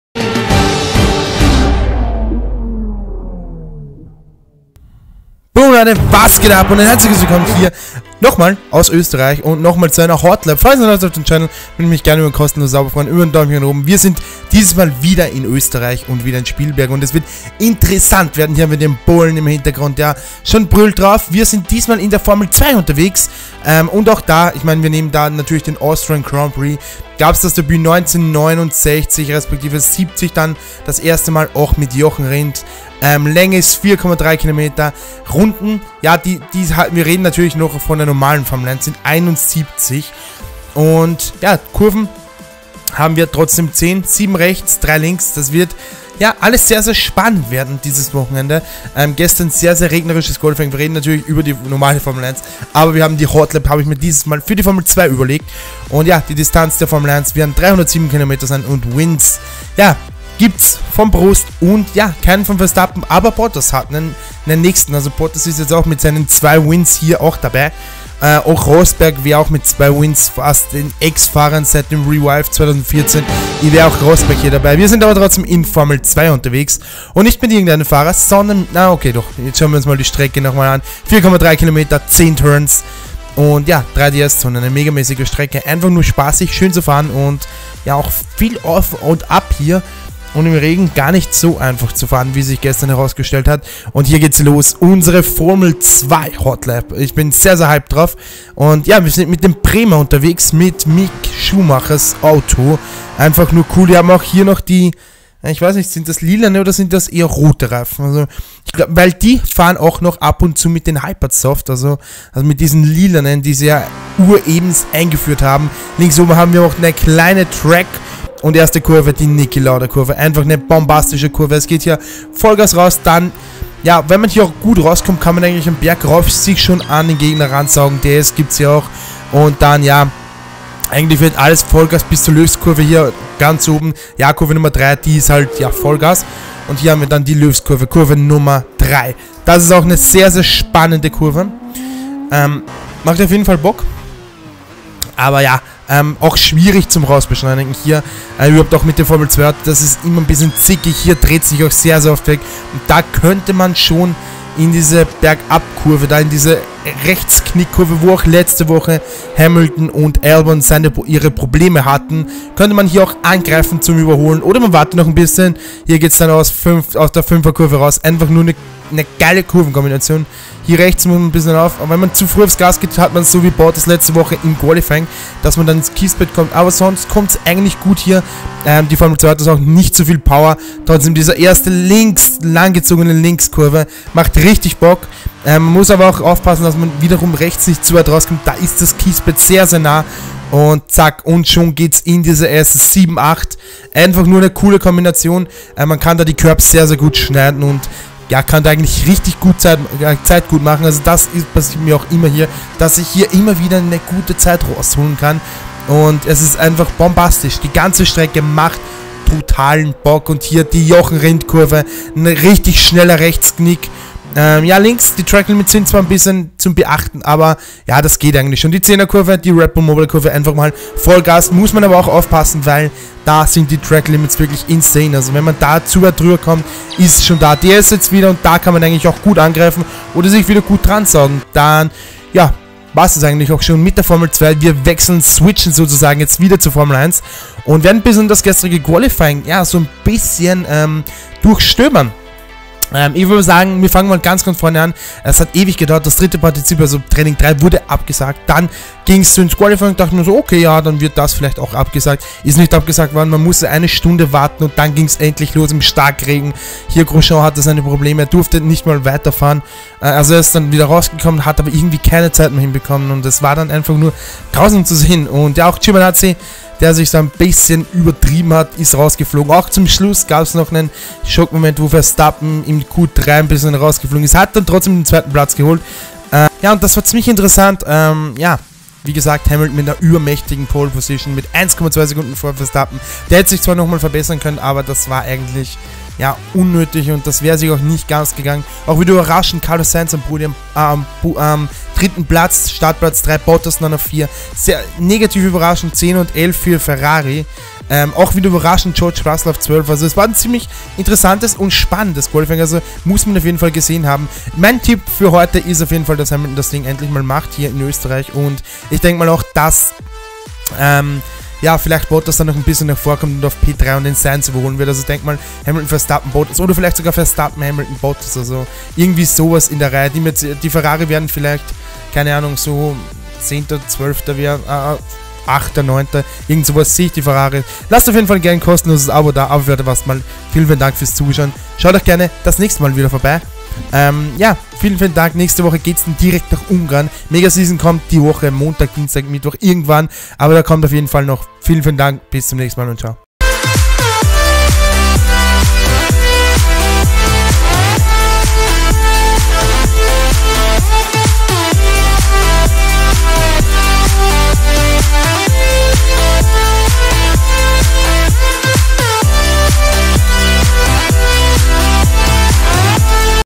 2020 Austrian Grand Prix: F1 - HOT LAP 2020 | AUTGP Grand Prix | AustrianGP StyriaGP #03
Willkommen aus Spielberg vom Red-Bull-Ring, im Video erkläre ich euch alles wichtiges, und alle wichtigen PUNKTE der Strecke!!! +Prognose von mir wer GEWINNEN wird!

Wir hoffen natürlich, dass es euch gefallen hat und würden uns über eure Unterstützung freuen. LINKS sind alle hier ⬇️